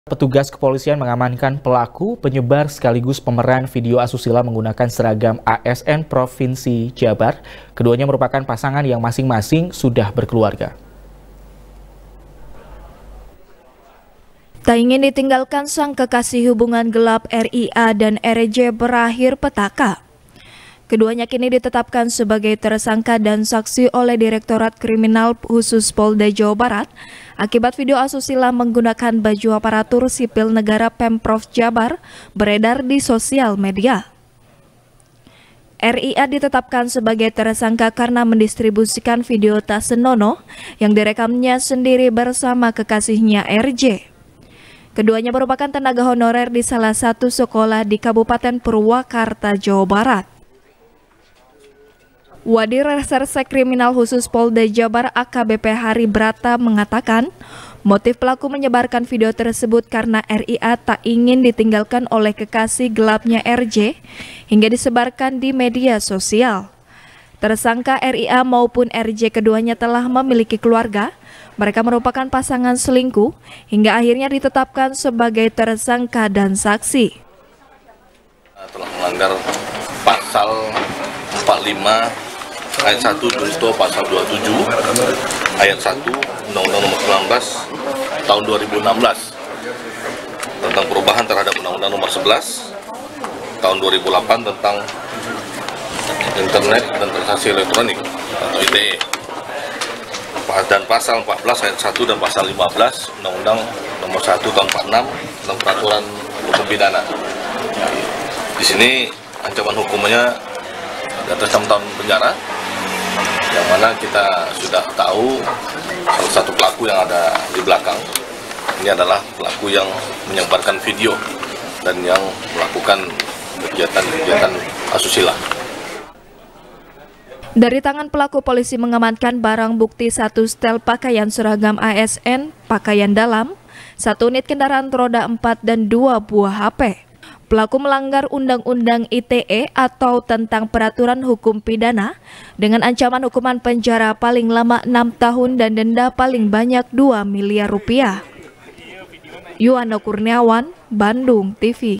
Polda Jabar Bekuk Penyebar Dan Pemeran Video Mesum Berseragam ASN | SBR | BANDUNGTV
SBR | BANDUNGTV | IRFAN RAMANDA | Petugas Kepolisian Mengamankan Pelaku Penyebar Sekaligus Pemeran Video Asusila Menggunakan Seragam ASN Pemprov Jabar. Keduanya Merupakan Pasangan Yang Masing-Masing Sudah Berkeluarga.